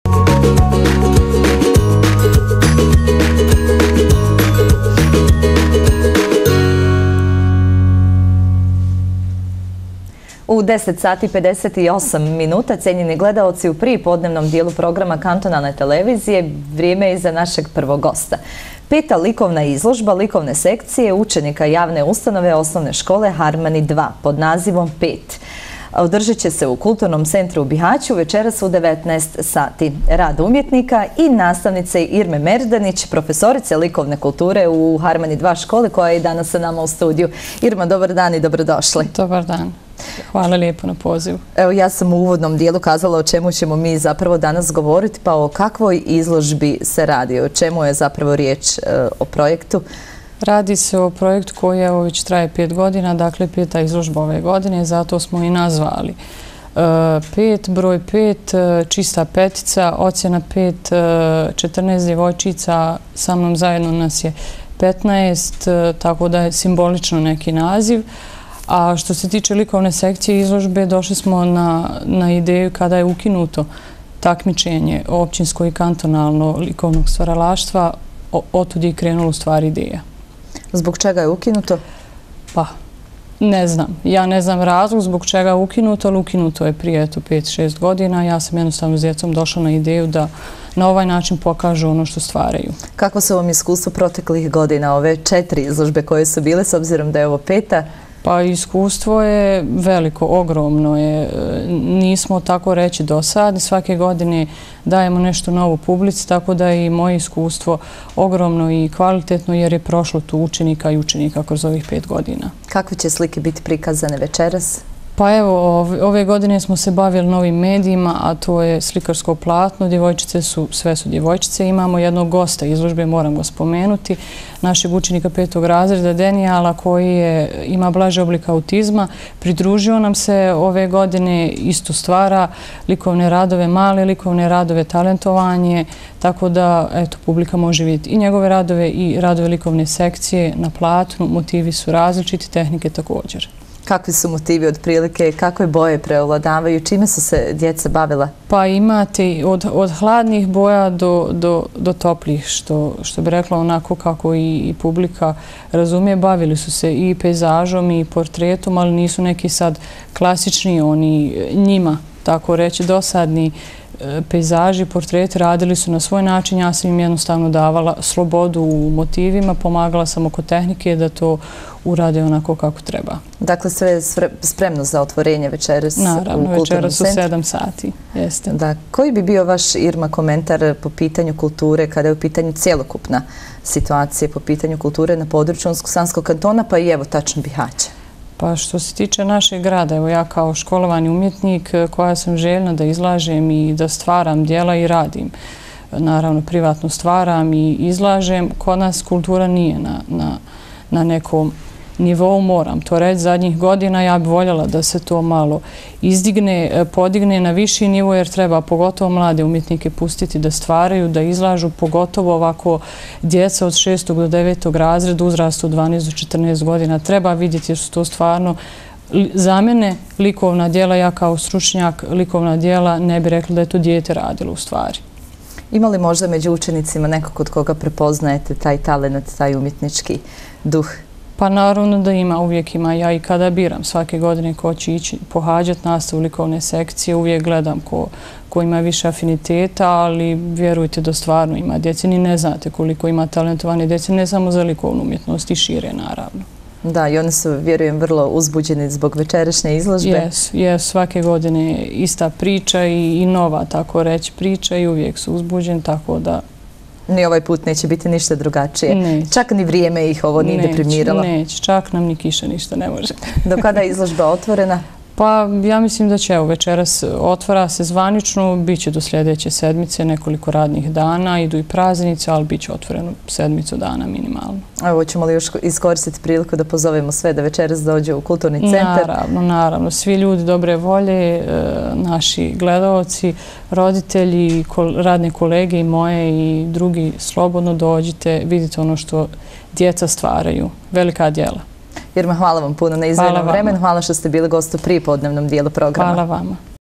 OD 10 DO 12: Peta likovna izložba Likovne sekcije učenika JU OŠ "Harmani II" pod nazivom "5".
U sali Kulturnog centra u Bihaću, večeras u 19h biće otvorena Izložba učenika OŠ "Harmani 2" i nastavnice Irme Merdanić. Izložba će biti otvorena nekoliko dana a svemu ovome opširnije možete čuti u razgovoru.